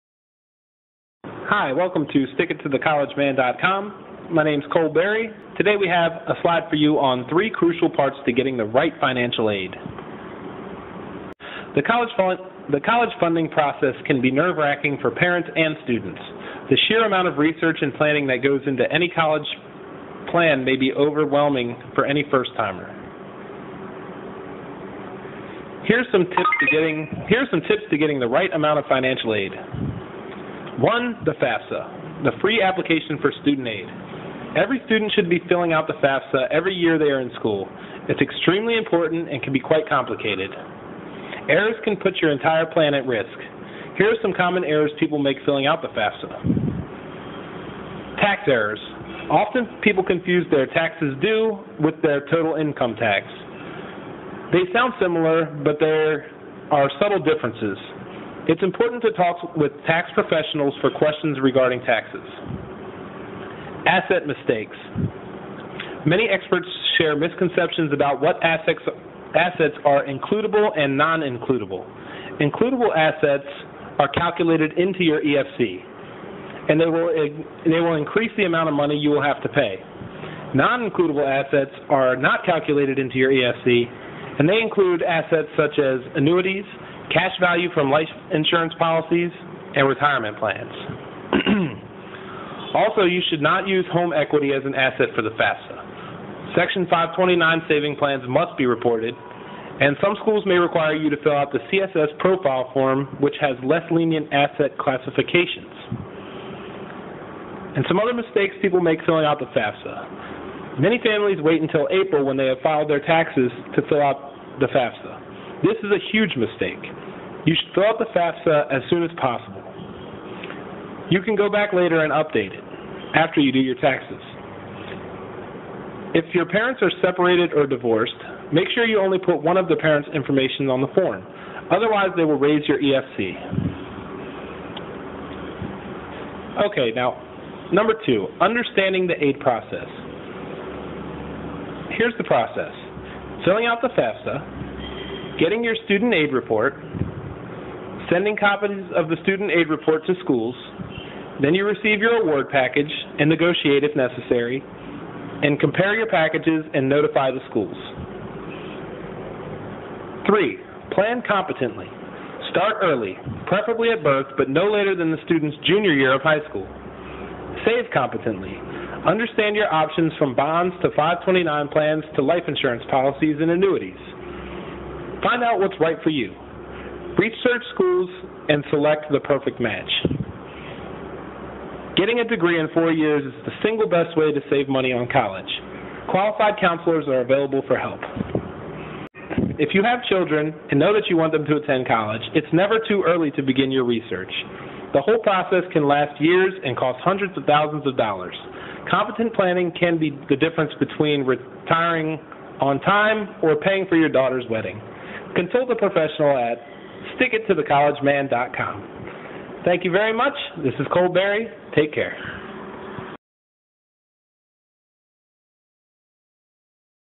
3 Crucial Parts of Getting the Right Financial Aid
Cole tells us the more important aspects of getting awarded the proper amount of financial aid.